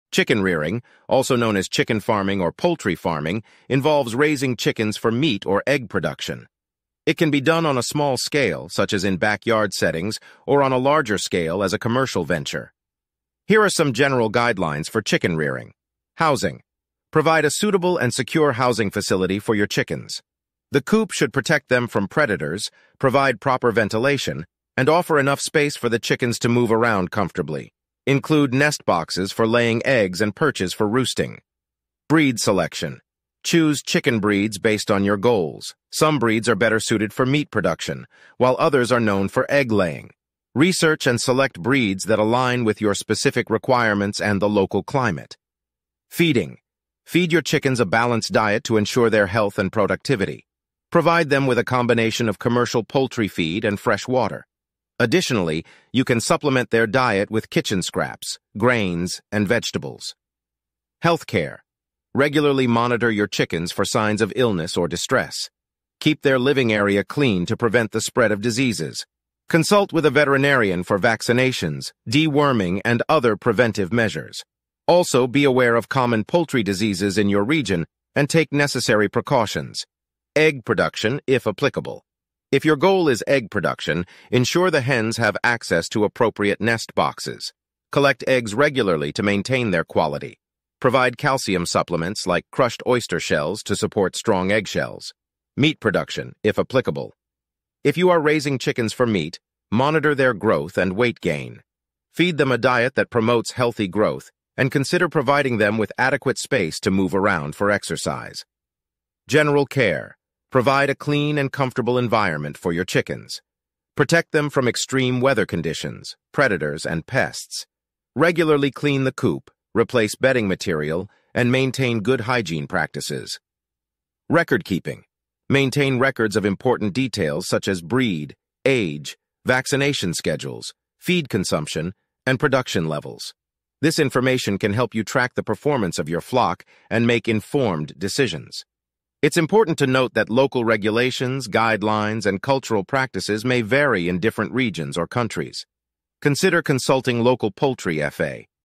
Mastering Chicken Rearing: Expert Tips
In this insightful video, unlock the secrets of successful chicken rearing with expert tips. Discover proven techniques for sustainable agriculture, breed selection, coop setup, and more. Elevate your chicken farming skills and create a thriving, healthy flock. Join us on a journey of mastering chicken rearing and gain the knowledge to raise chickens like a pro.